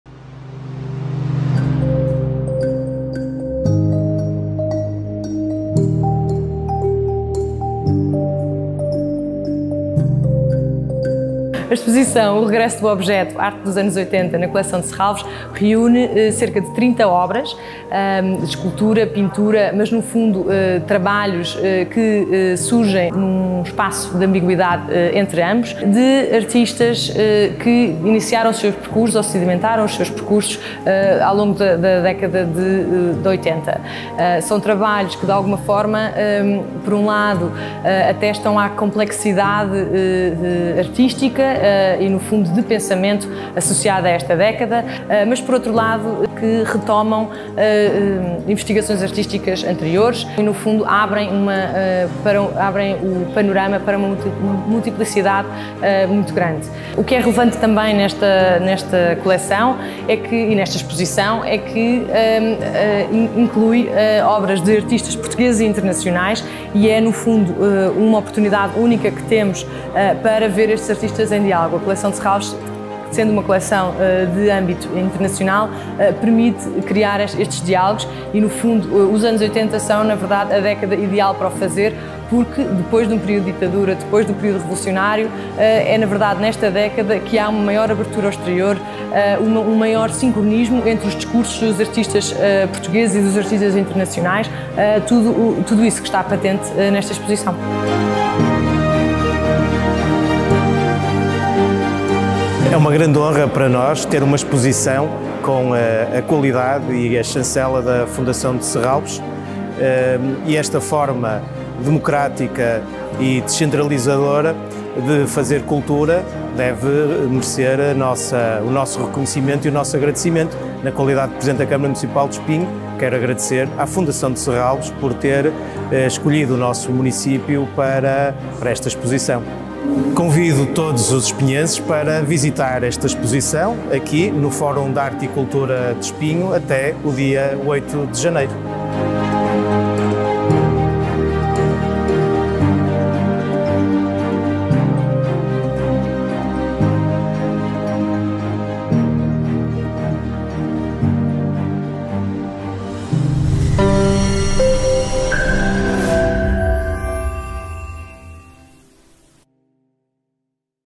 Inauguração da Exposição "O Regresso do Objeto: Arte dos Anos 1980 na Coleção de Serralves"
Foi inaugurada, na passada quarta-feira, dia 27 de Outubro, no Fórum de Arte e Cultura de Espinho a exposição "O Regresso do Objeto: Arte dos Anos 1980 na Coleção de Serralves".

Esta exposição apresenta trabalhos de artistas portugueses e estrangeiros que sedimentaram os seus discursos artísticos nos anos 80.

As obras apresentadas personificam as transformações a que a arte foi sujeita nesse período e revelam como a complexidade das práticas destes artistas excede as ideias preconcebidas sobre a arte dos anos 80.

Exposição patente até dia 8 de janeiro de 2022, na Galeria Amadeo de Souza-Cardoso - Museu Municipal de Espinho.

Artistas representados:
Rui Aguiar, Joaquim Bravo, Gerardo Burmester, António Campos Rosado, José Pedro Croft, Pedro Cabrita Reis, Rui Chafes, Ana Jotta, Harald Klingelhöller, Juan Muñoz, Rui Sanches, entre outros.